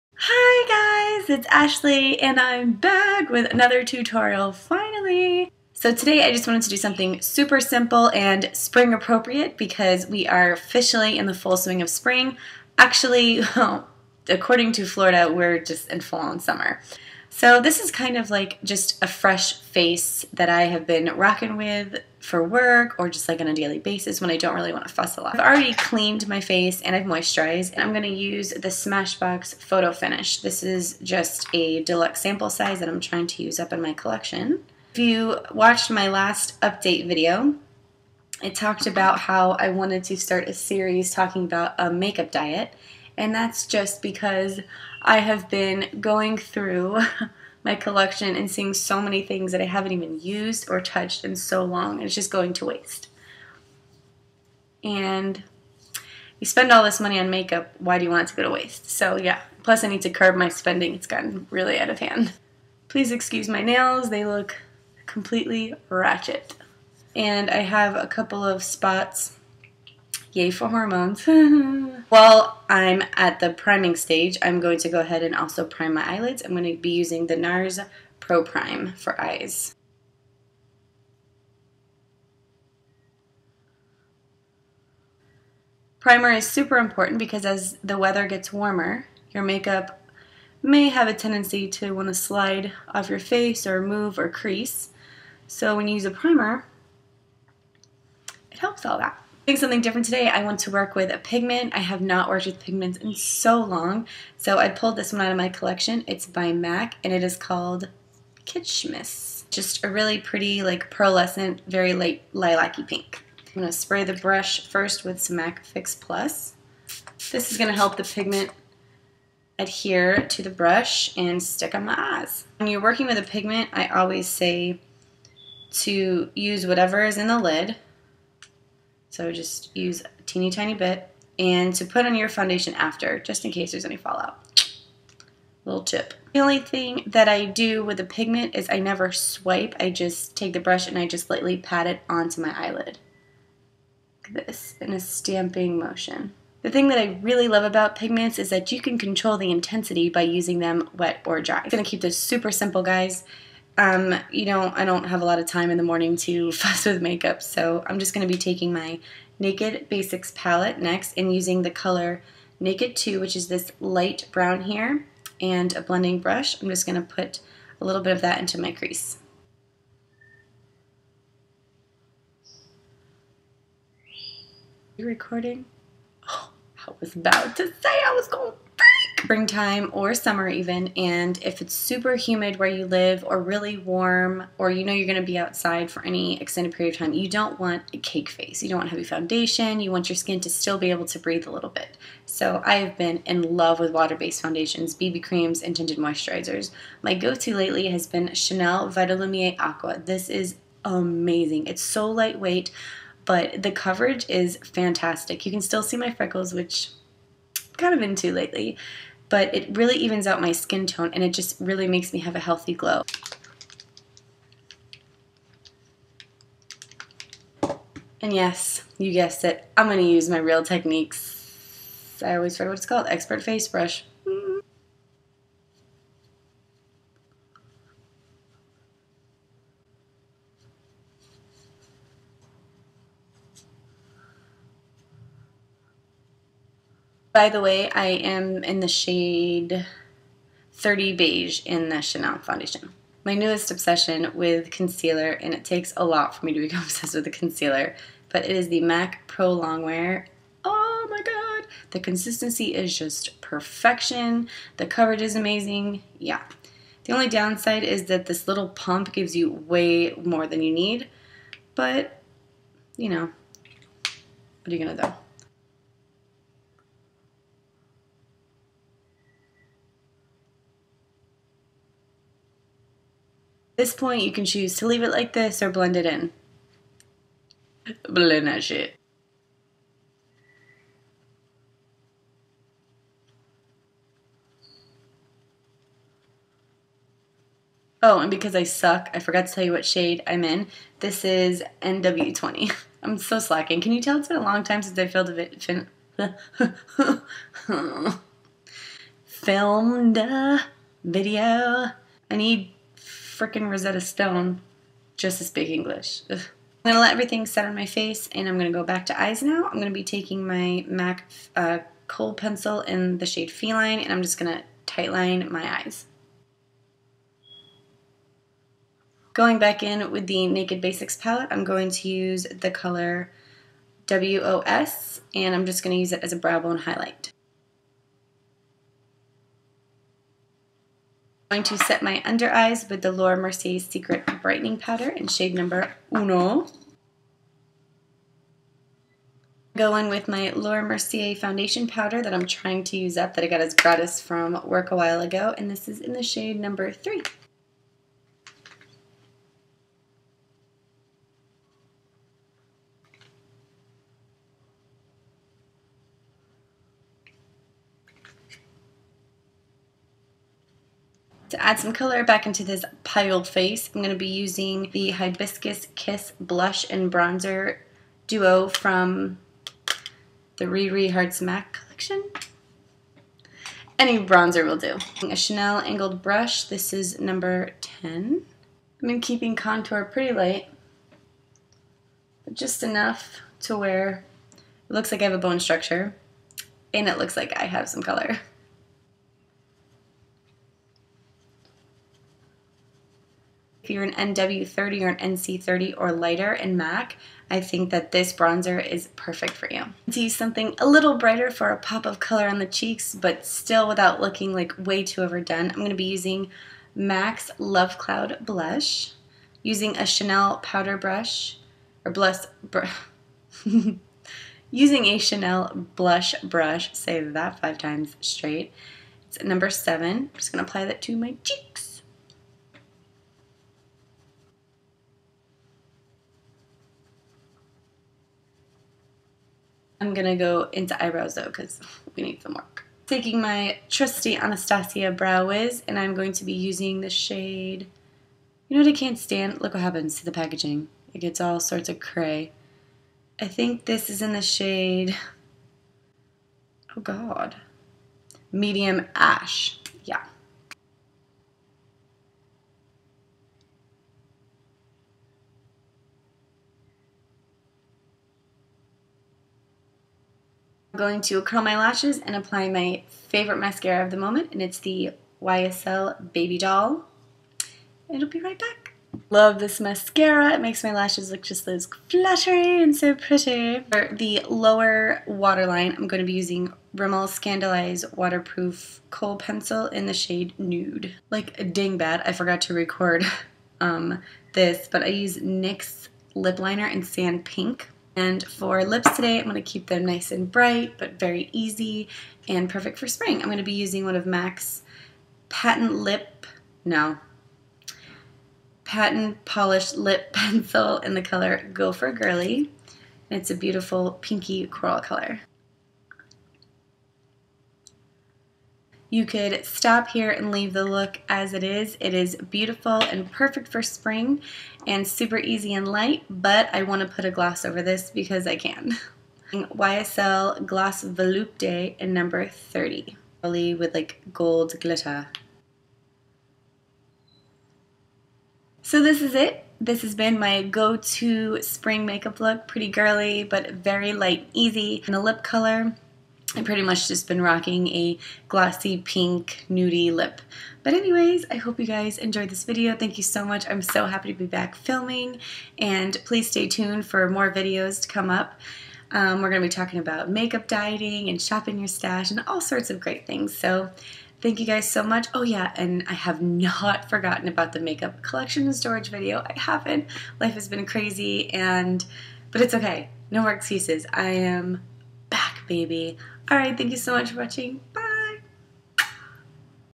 My everday Spring makeup look! Fresh faced and easy ;)
Hello friends!!! It's that time again, so grab a snack and let's chat and get ready together!

XO~Ashley